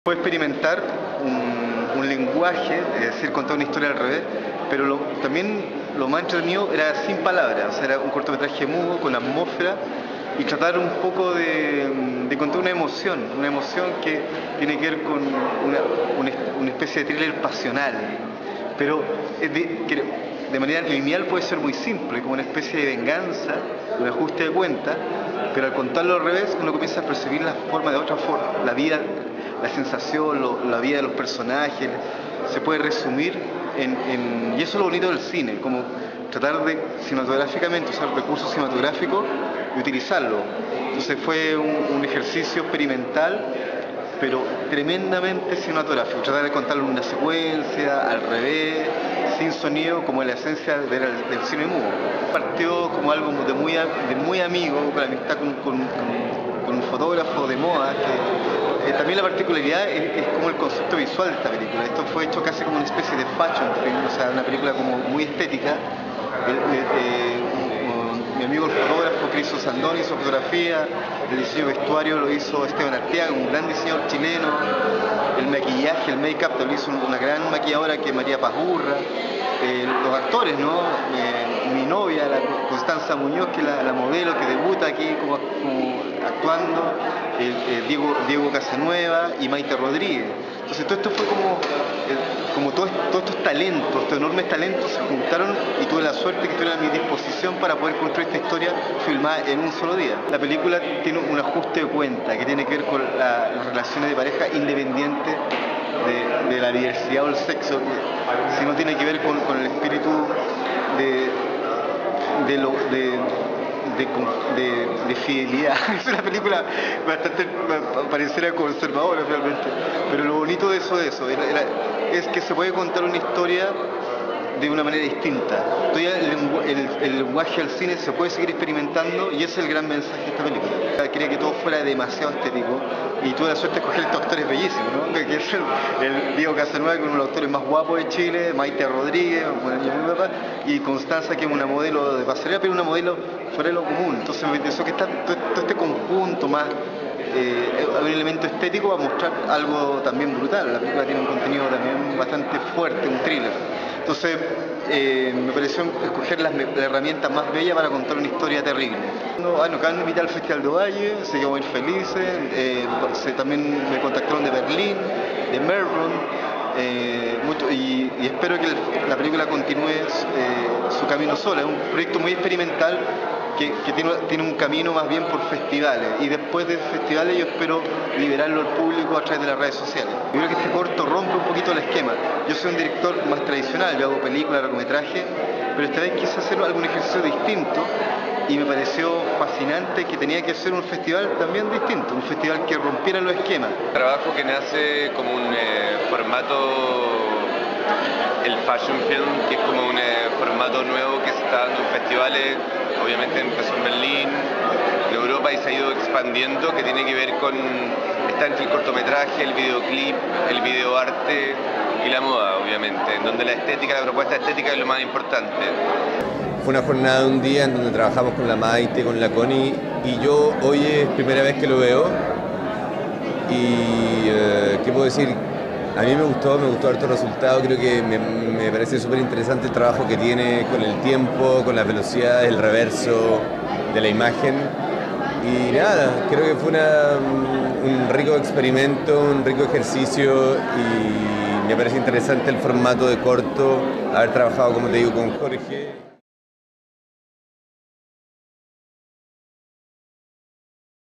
[CINE CHILENO] Jorge Olguín y Diego Casanueva hablan sobre el cortometraje "Reverso"
Estrenado el pasado domingo 15 de noviembre, "Reverso" es un cortometraje experimental que se inserta en la categoría del "fashion film", en donde los recursos estéticos juegan un rol fundamental.

Dirección: Jorge Olguín; Elenco: Diego Casanueva, Mayte Rodríguez y Constanza Muñoz.